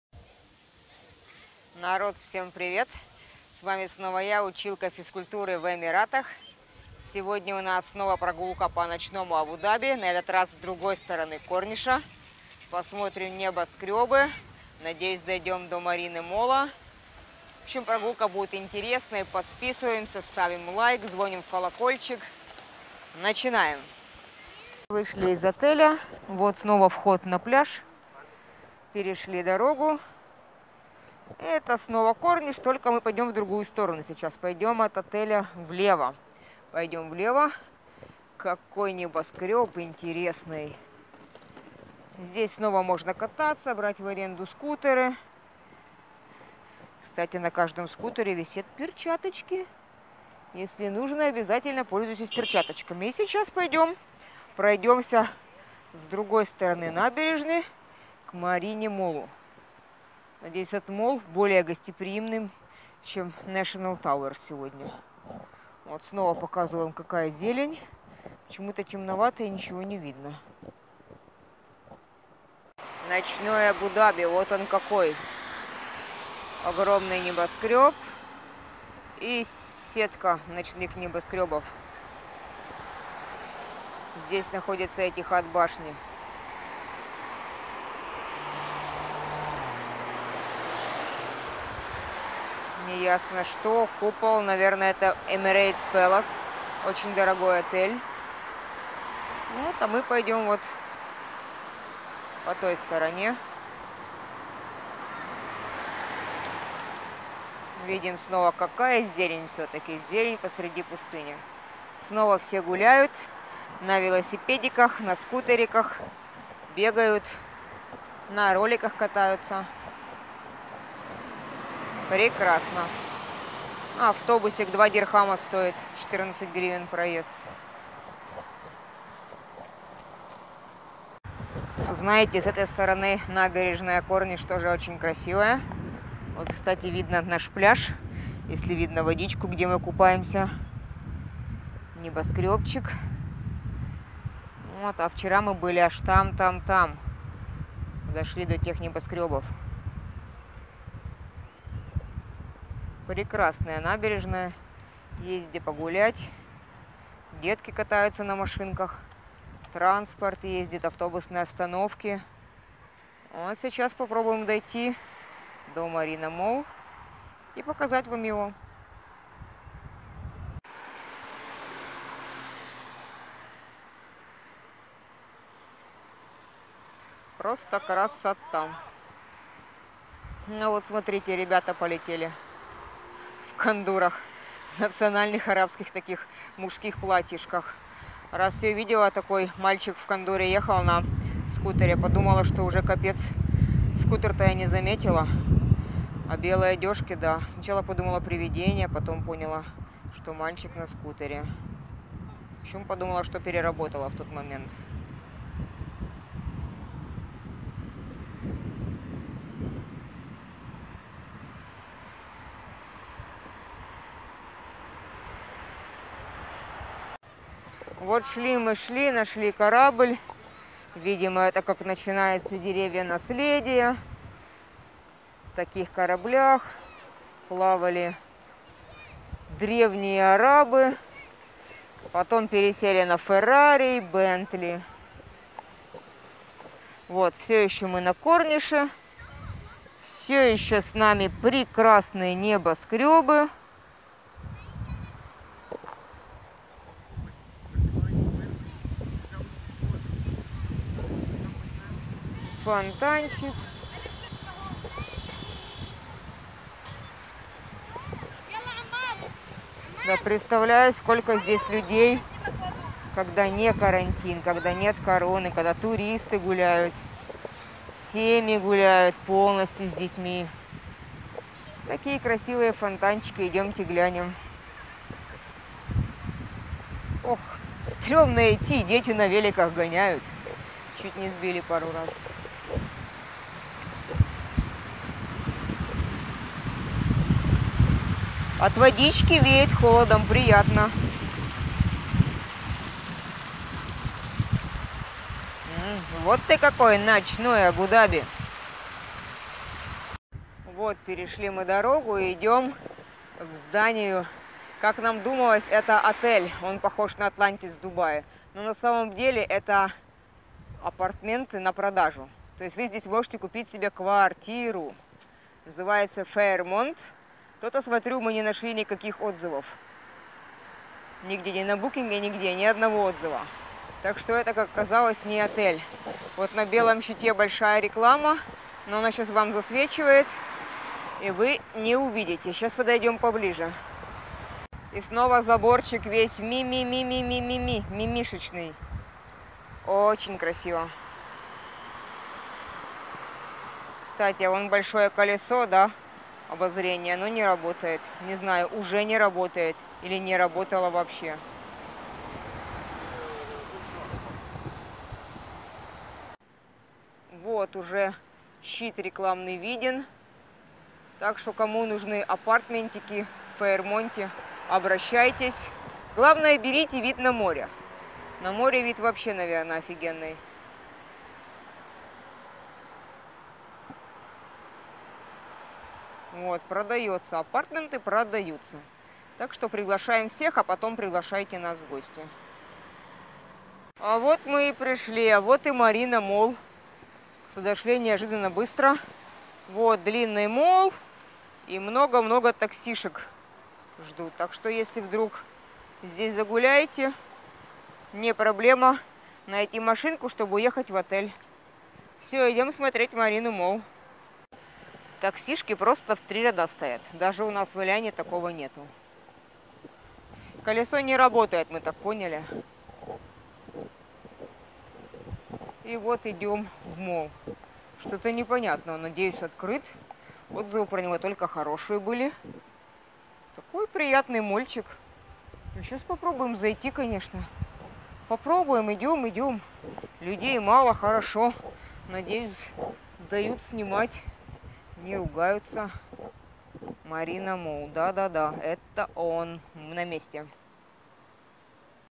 ДРУГАЯ СТОРОНА КОРНИША, АБУ-ДАБИ, ИДЕМ НАЛЕВО И ПОКАЗЫВАЕМ ВСЕ!
Всем Привет!
Сегодня вечерняя прогулочка в другую сторону Набережной Корниш в Абу-Даби.